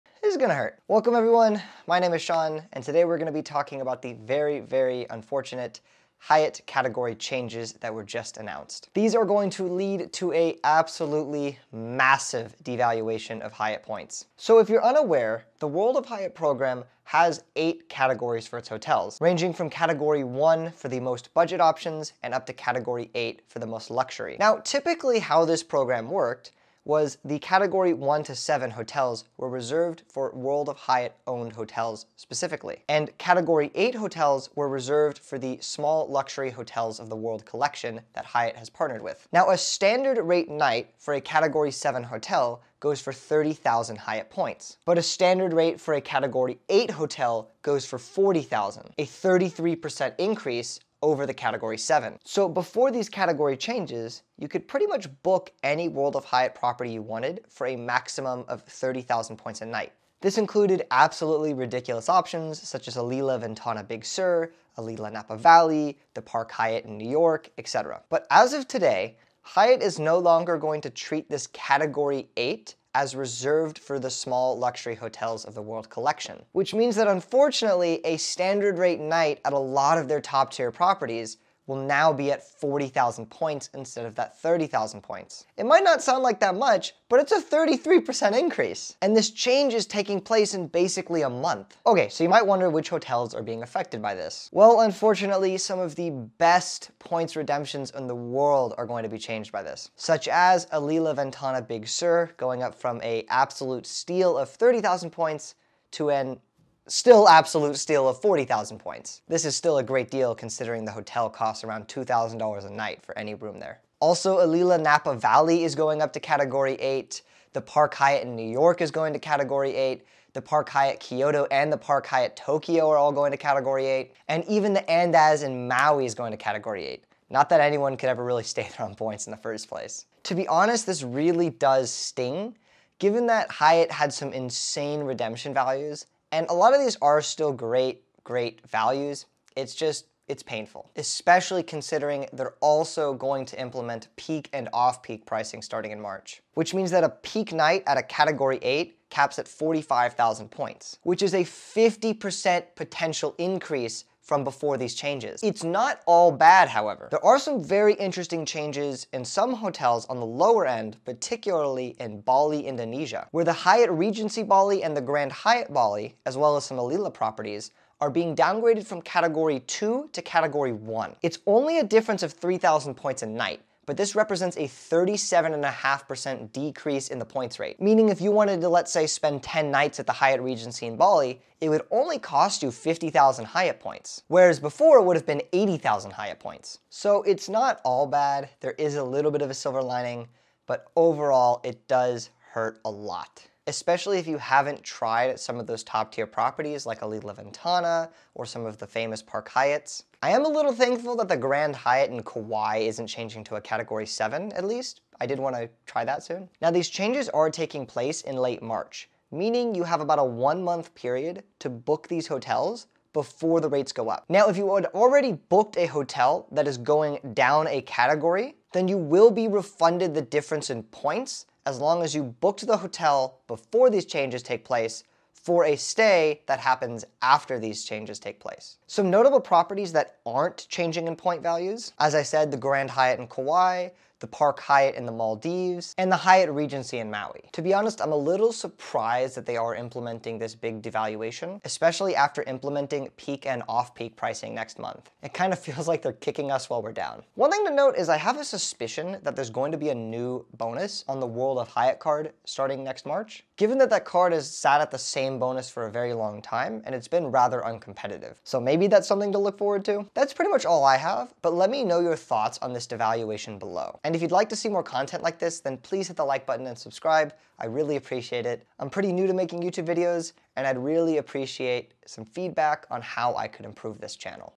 Hyatt MASSIVE Category Devaluation 2022
UPDATE: The Park Hyatt Tokyo did *not* go up in category. I am unsure where I heard it was going up but I was mistaken. The Park Hyatt Tokyo is remaining at category 7.

Hyatt just announced its category changes for March 2022 and it is going to cause an absolutely monstrous devaluation of Hyatt points.

A bunch of top tier Hyatt properties are moving into category 8 which is a minimum of a 33% increase in point cost.

Luckily, the news isn't all bad since there are a lot of hotels going down a category.